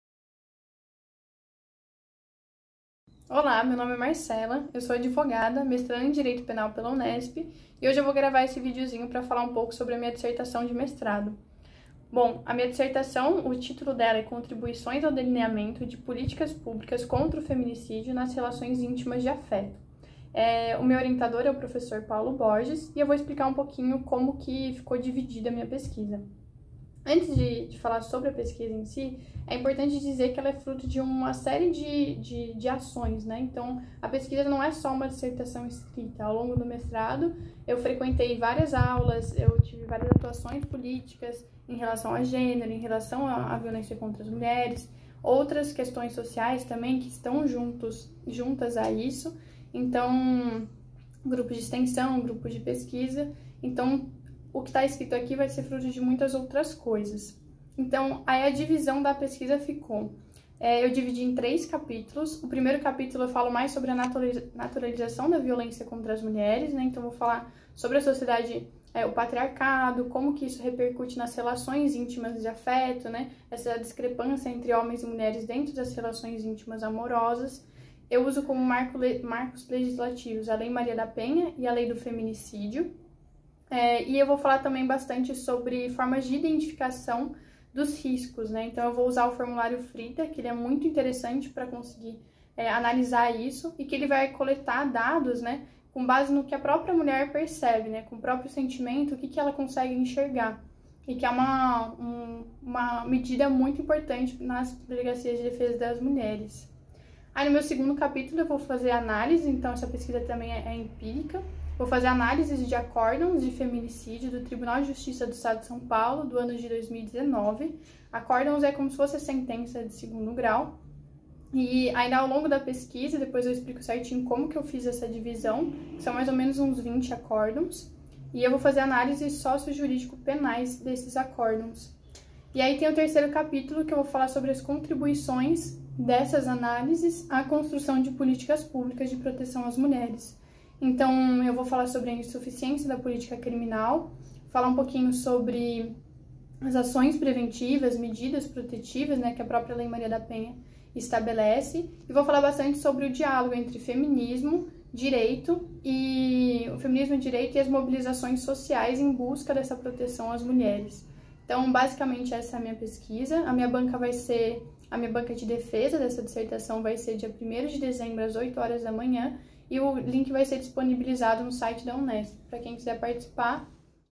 Minha Tese em 3 minutos - #4
Minha tese em três minutos - Unesp Franca

Minha tese em três minutos é um novo projeto da Unesp com o objetivo de trazer convidados mestrandos e doutorandos da FCHS para falar sobre suas pesquisas.

No vídeo, a discente Marcela Helena Petroni Pinca, do Programa de Pós-Graduação em Direito fala sobre a sua dissertação, desenvolvida sob a orientação do Prof. Paulo César Correa Borges.